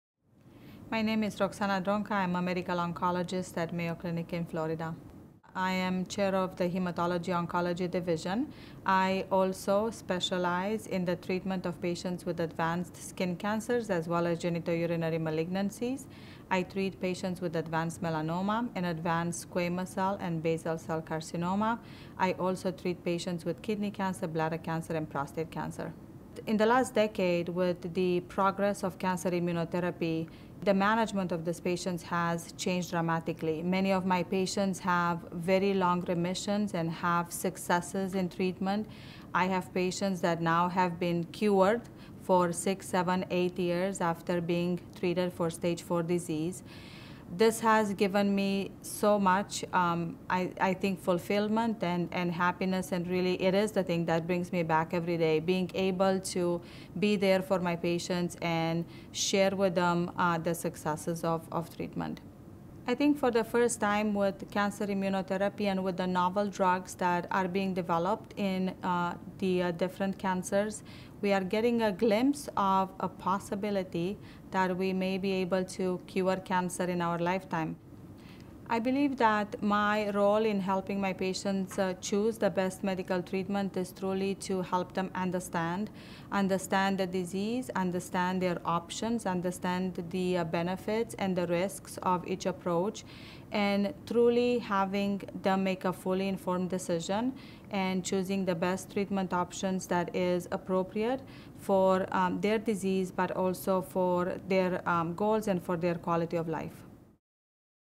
Dr. Roxana Dronca, M.D.: Oncologist - Mayo Clinic
Dr. Roxana Dronca discusses the treatment of advanced skin cancer and advanced genitourinary malignancies. She also highlights the transformative progress of immunotherapy in cancer treatment.